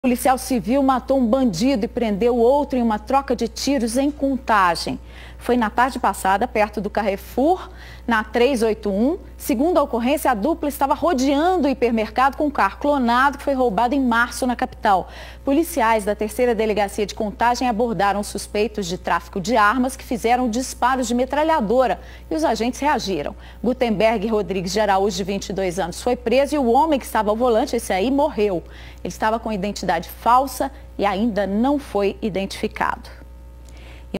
Policial troca tiros, mata bandido e prende outro
Na tarde dessa quarta, um policial matou um bandido e prendeu outro durante uma troca de tiros. O caso aconteceu próximo ao Carrefour de Contagem, na BR381. Segundo a ocorrência, a dupla estava num carro clonado e rodeava o estabelecimento. Os policiais da 3ª Delegacia de Contagem abordaram os suspeitos de tráfico de armas que fizeram os disparos. Durante a troca de tiros, Gutemberg Rodrigues de Araújo (22) foi preso. O outro homem, que estava ao volante, morreu na hora. Ele estava com identidade falsa.